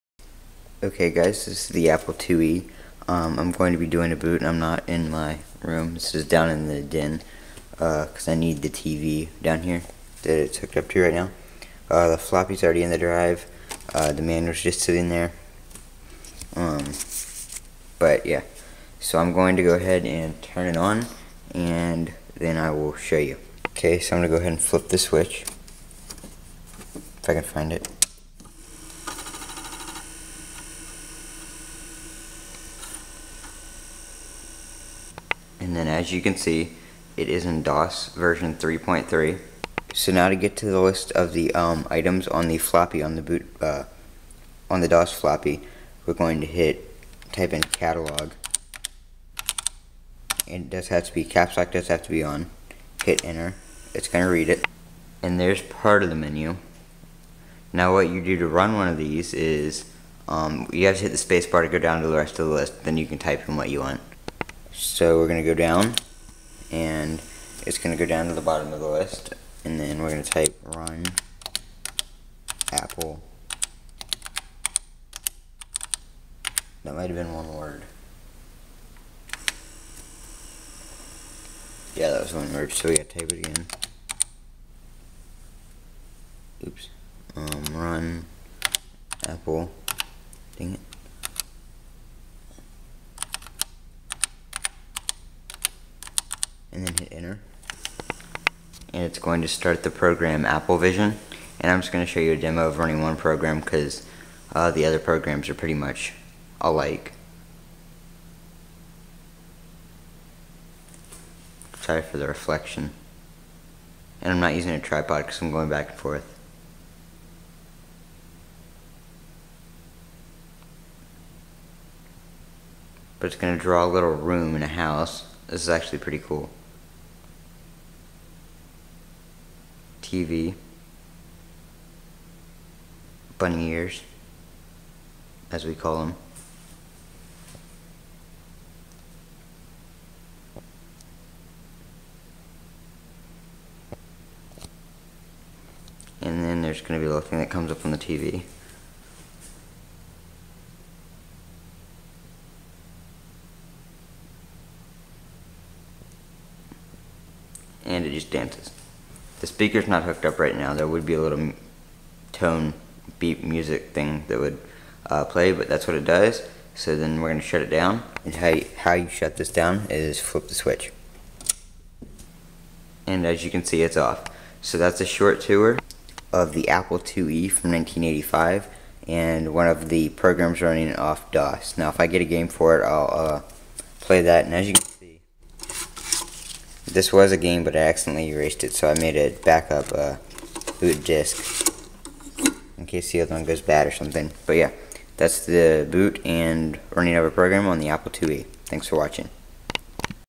Apple IIe Boot DOS 3.3/Run a Program
Code: Techout I joined Maker Studios & so can you!

Booting into DOS 3.3 and running a program on an apple IIe